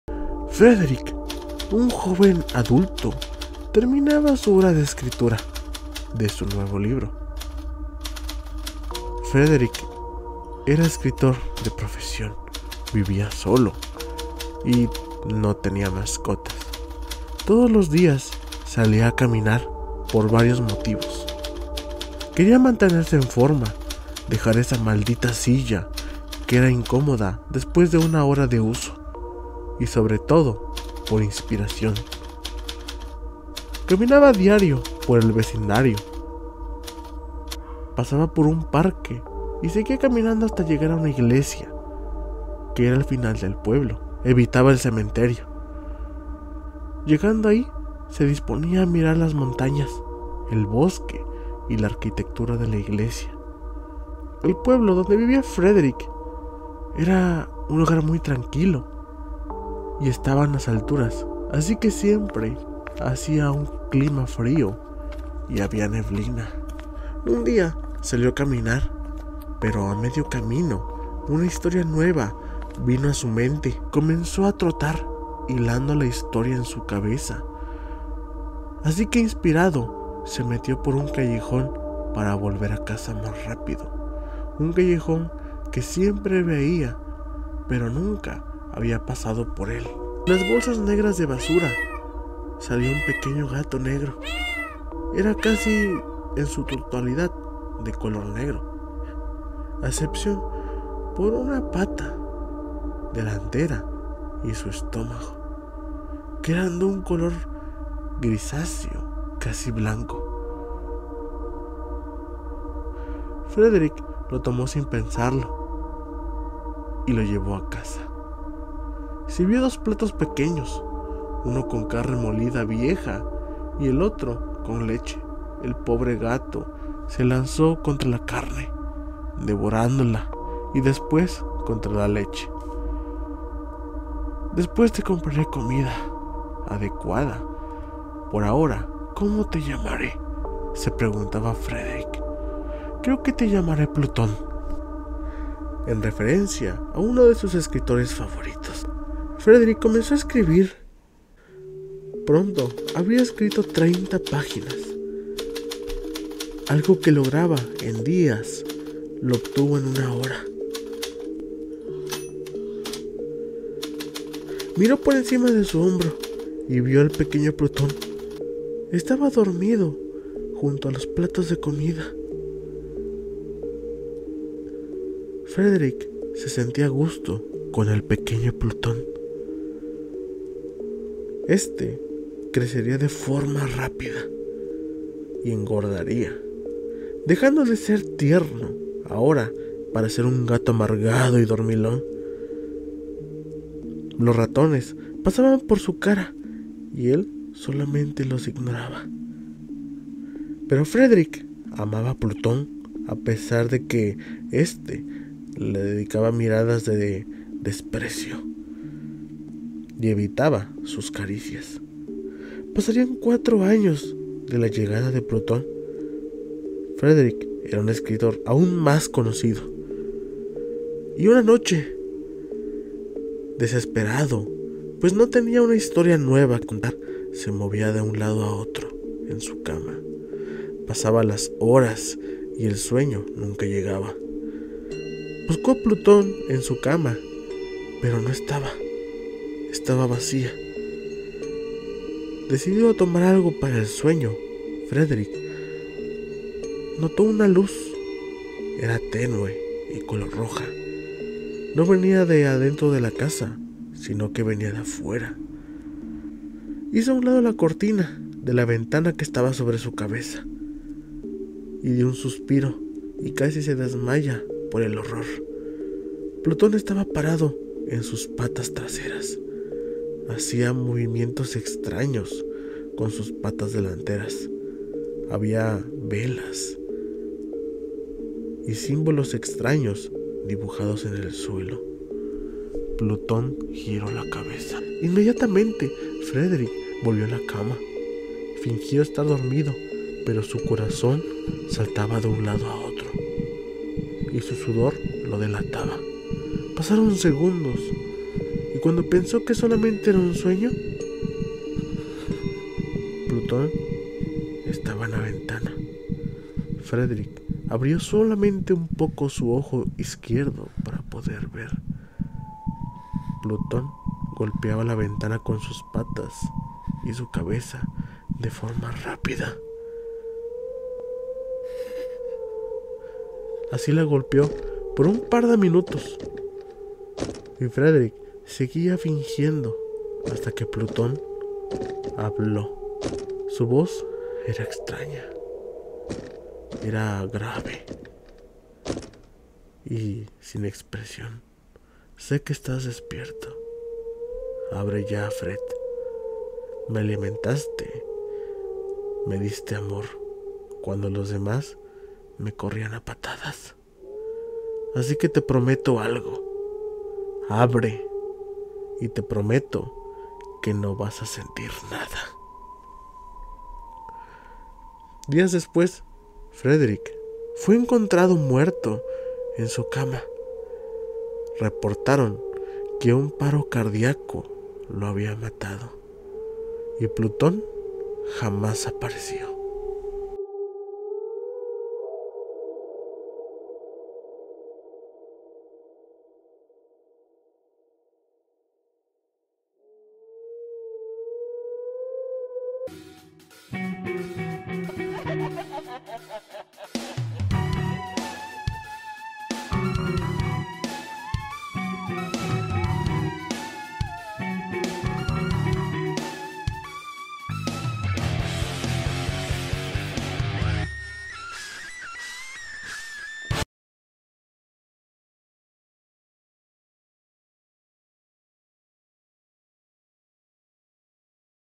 El gato que habló / Historia de terror
Todos o la gran mayoría hemos tenido mascotas, y todos sabemos que los gatos y los perros son los mas populares. Ahora aquí te cuento que pasaría si tu gato hablara, y todos tenemos ese miedo de verlo parado en sus patas traseras y verlo hacer cosas de humanos, como hablar.